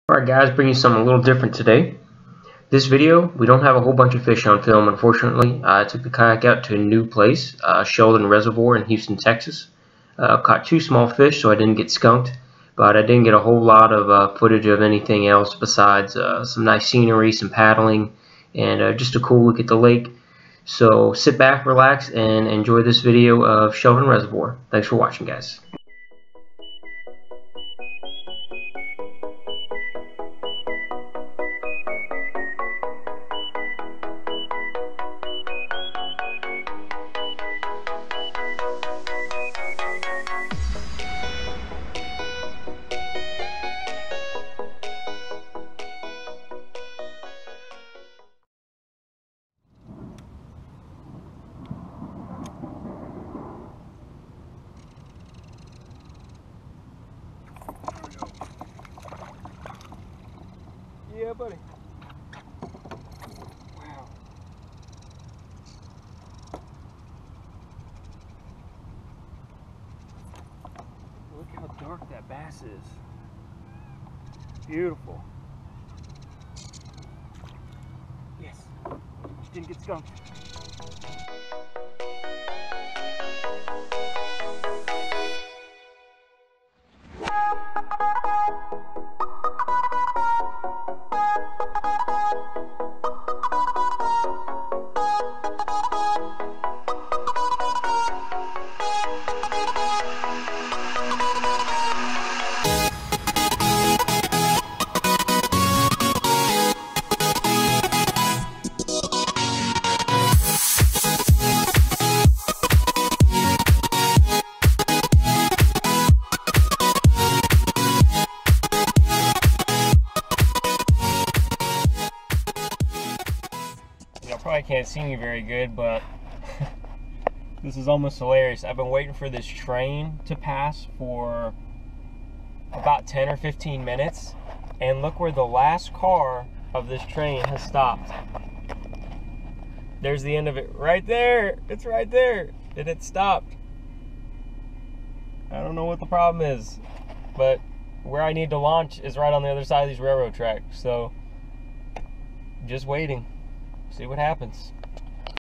Kayak Bass Fishing Sheldon Reservoir, TX | I almost got skunked.
Took the kayak out this past Sunday to a lake that's been on my list for some time, Sheldon Reservoir in Houston, Texas.

First off I must say, this is the most beautiful lake I have ever been on or fished. The scenery and setting is unreal, you feel like you're on a lake 5 hours away from Houston tucked in the woods, yet it's right down the road from a lot of major power plants and chemical plants. I manged to catch 2 small fish, but that was it. Had some awesome blow ups, but no hookups on topwater. Overall, very pretty lake, there was a lot of anglers on the water and at the ramp, more than I was expecting for such a small body of water, I didn't realize this lake received so much fishing pressure. Not sure how soon I'll go back but I was happy to visit it and get on the water.

Enjoy and thank you all for your continued support